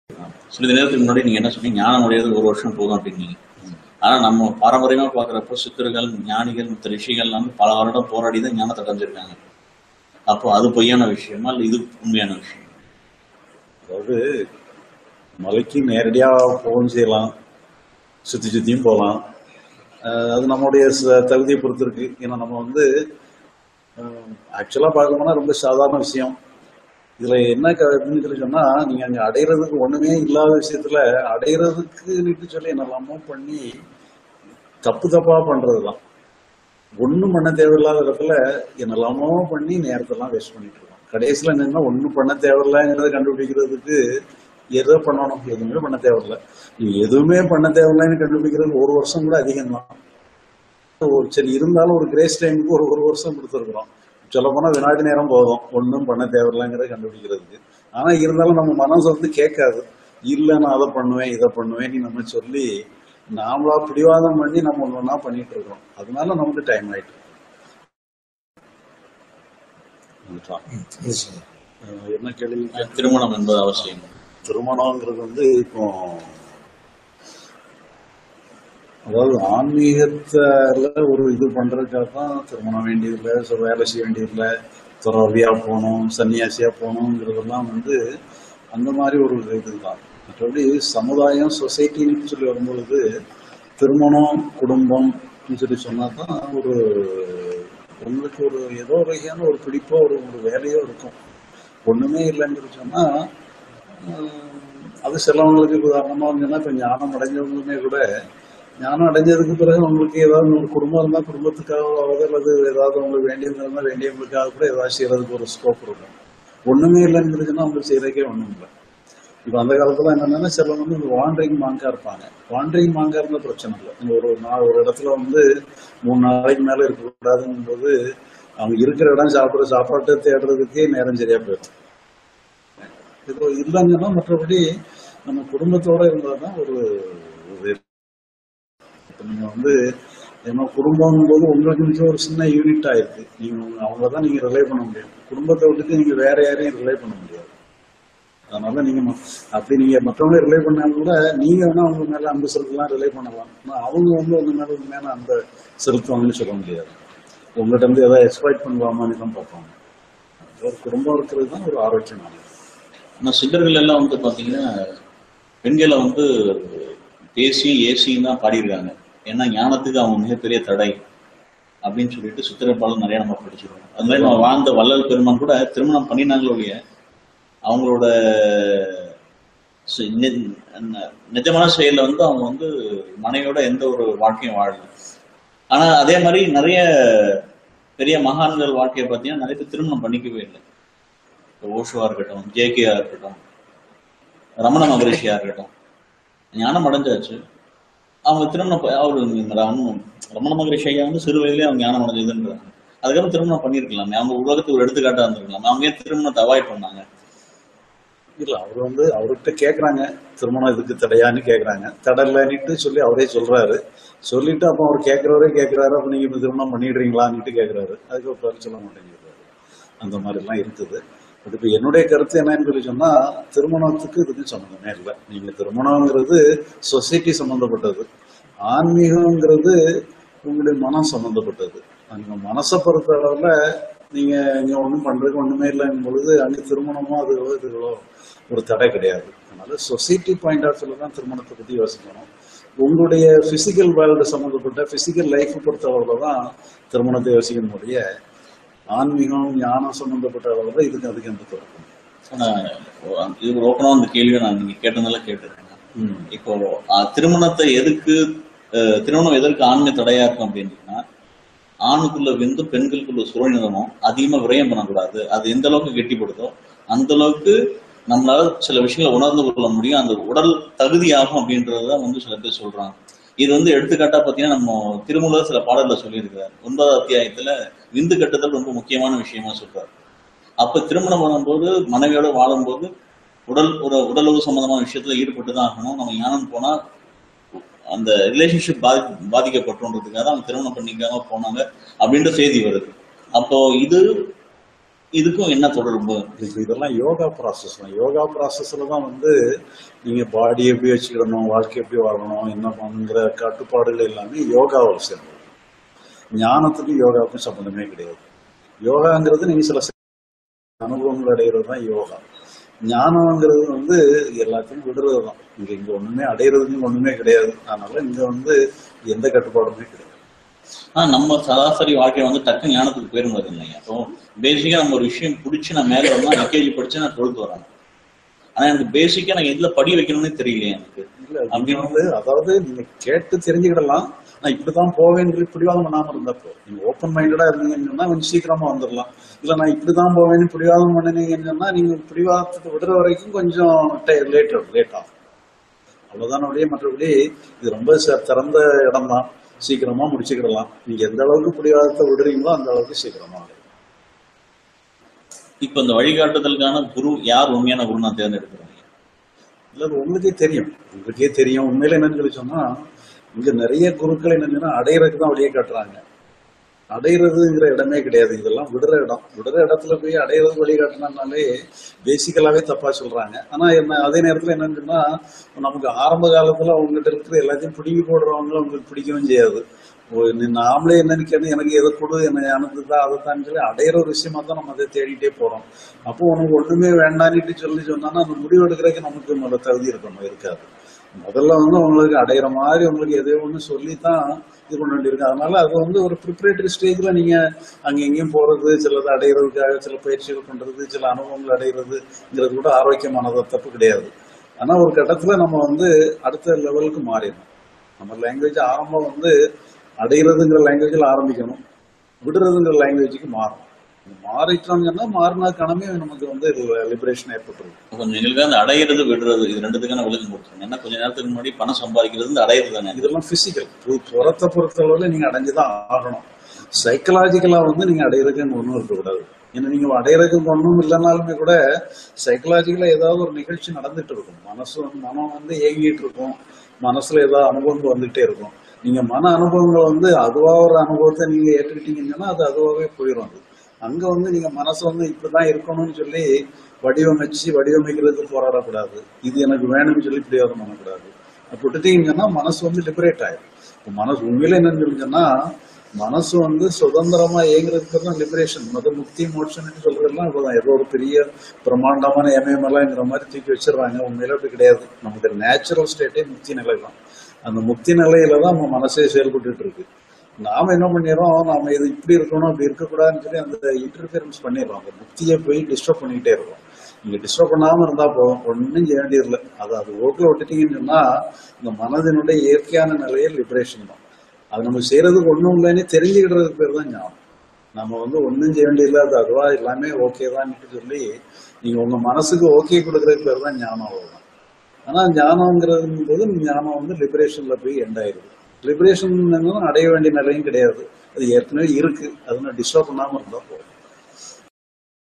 290718 திருமணம் Marriage - Q&A Sri Bagavath Tamil
Sri பகவத் ஐயா Introduction Whatsapp குழுவில் இணைய ஞானம் அடைவது என்பது சாதாரணமான விஷயம். திருமணம் என்பது வேண்டுமா ?  குடும்பத்துடன் இருப்பது சரி .  திருமணம் என்பது society சம்பந்தப்பட்டது.  திருமூலரின் விந்து கட்டுதல் பற்றிய கேள்வி.